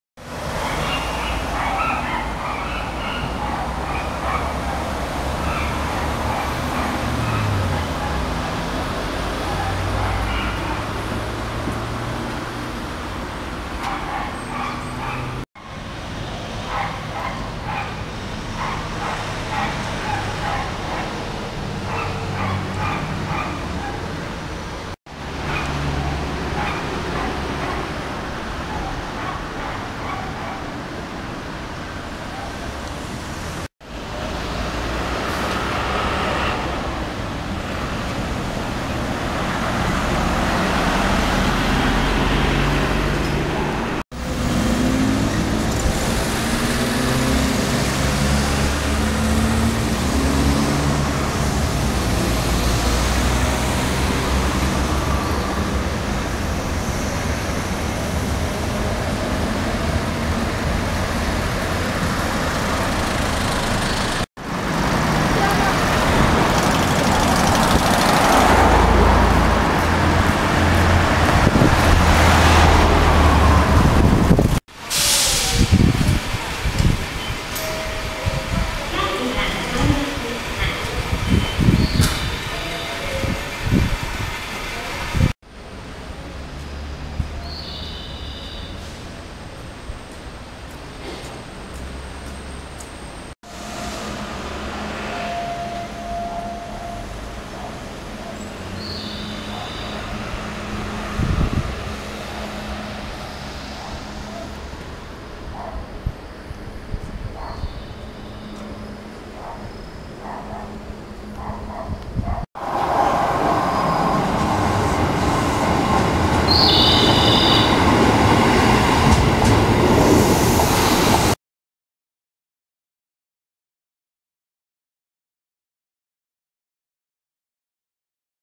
上溝駐輪場
上溝の駐輪場は1日100円で止められます。